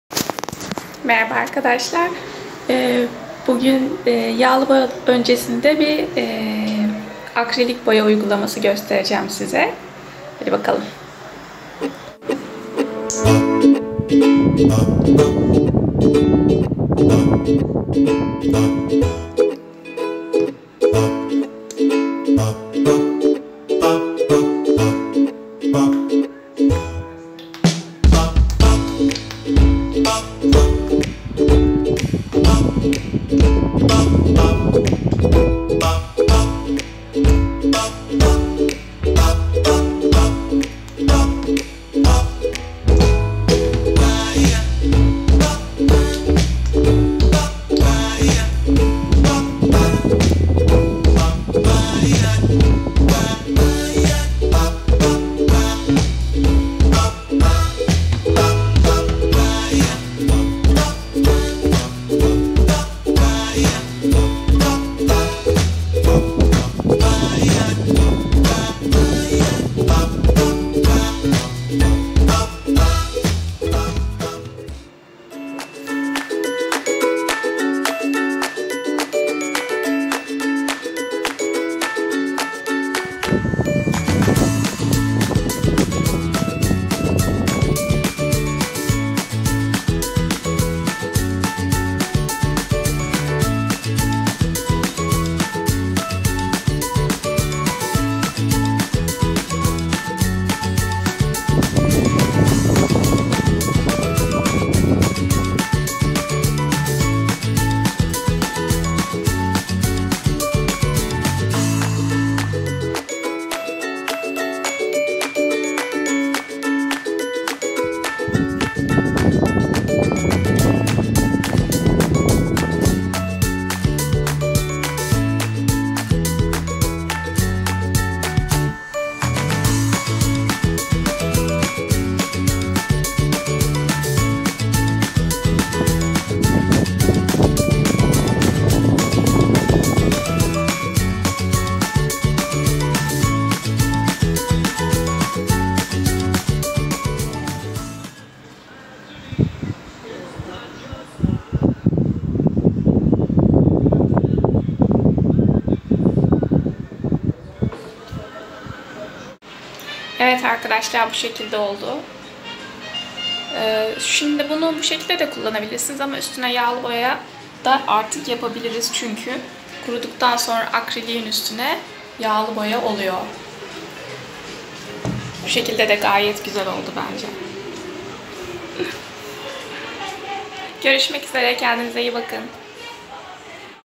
Akrilik Boya İle Resim - Etkinlik Önerisi
Merhaba arkadaşlar,
Bu videomuzda akrilik biya ile soyut bir resim yapıyoruz. Akrilik boyanın bu pratik kullanımıyla pek çok resim yapabilirsiniz.

Gökşen Parlatan